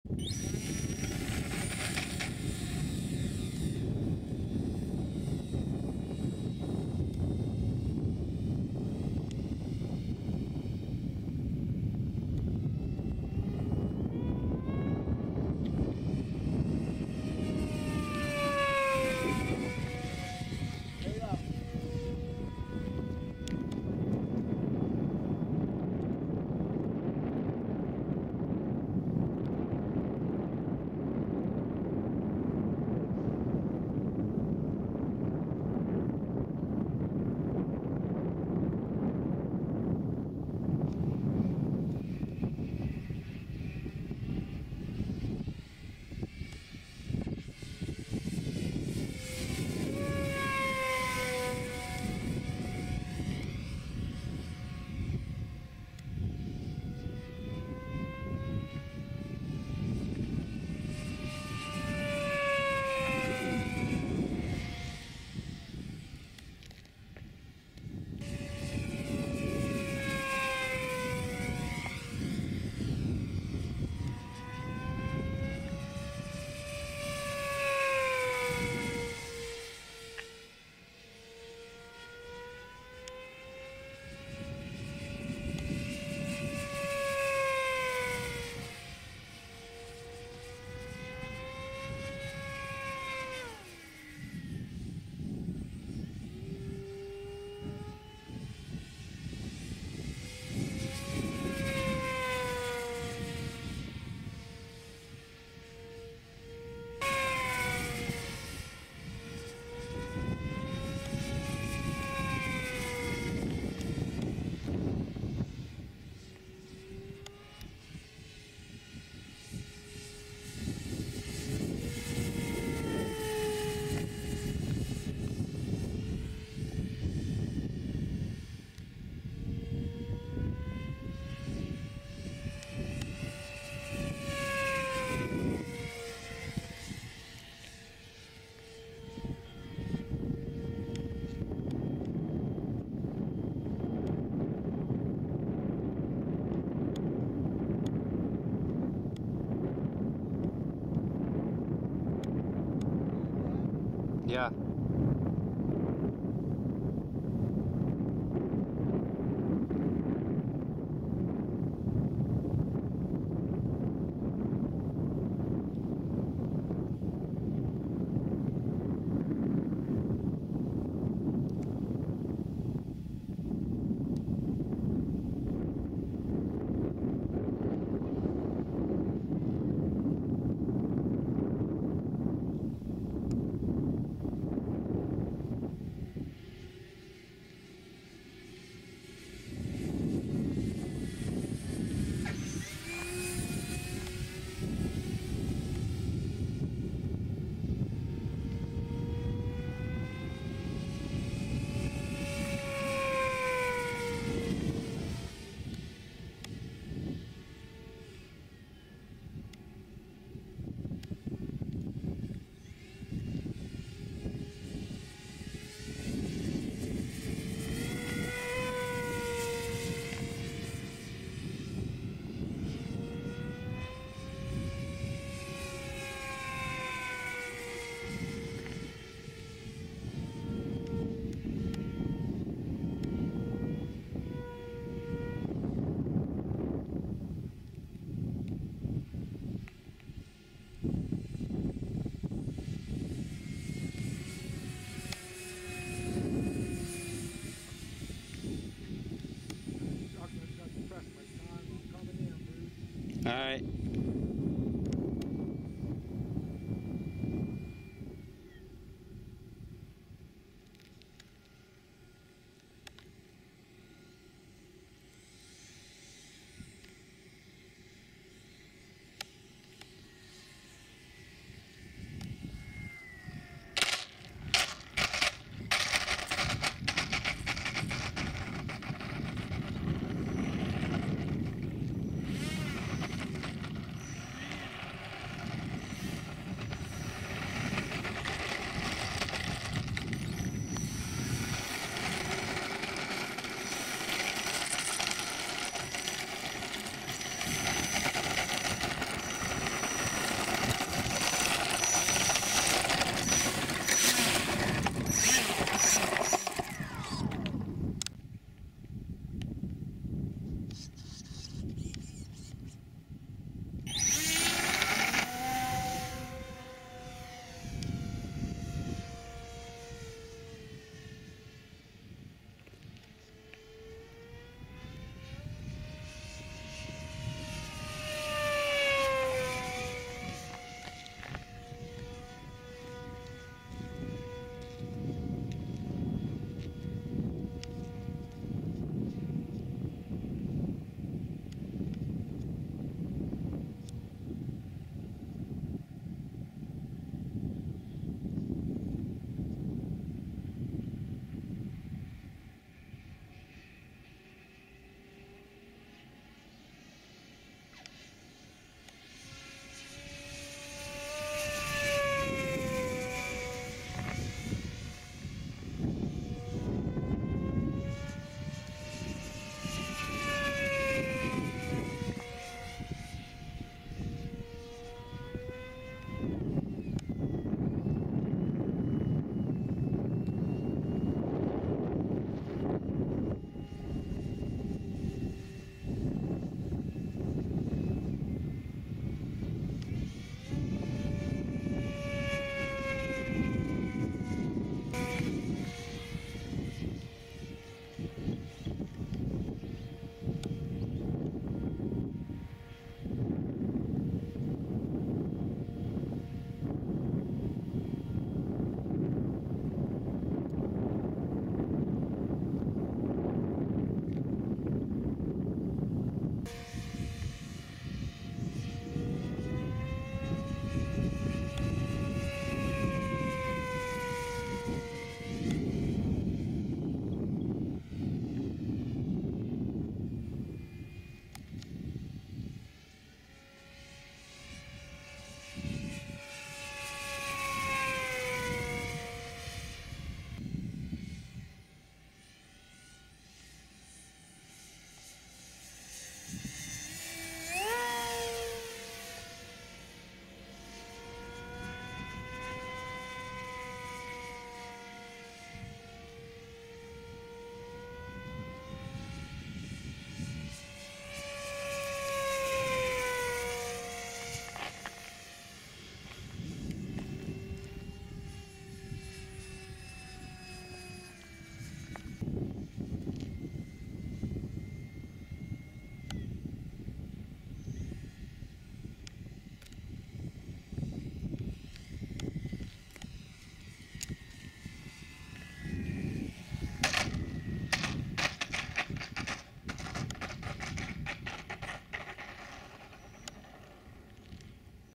Quickie 500 Electric Powered
Upgraded Power system.
+ a spinner and thinner wheels.